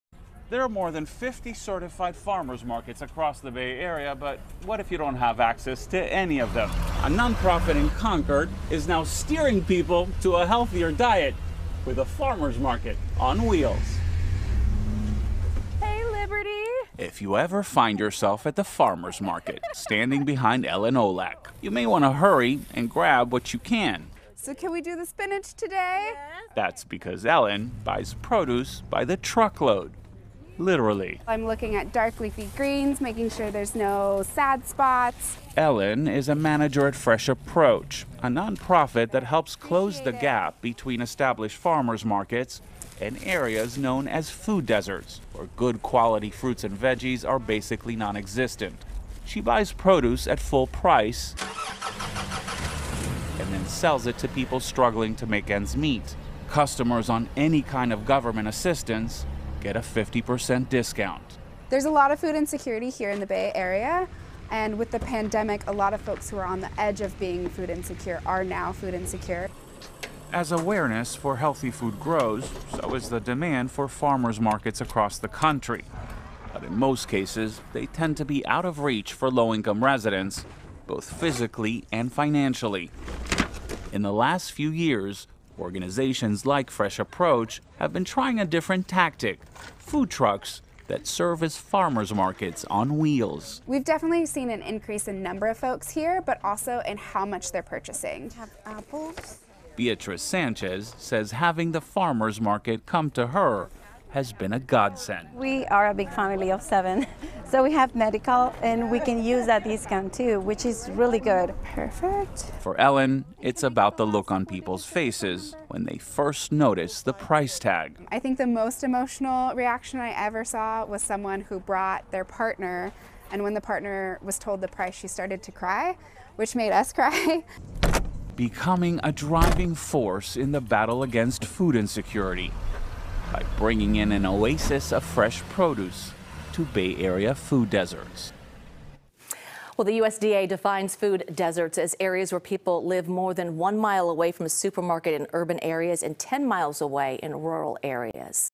Mobile farmers market brings fresh produce to Bay Area communities
A nonprofit organization called Fresh Approach is steering people to a healthier diet with a farmer's market on wheels. Itay Hod reports. (2-16-23)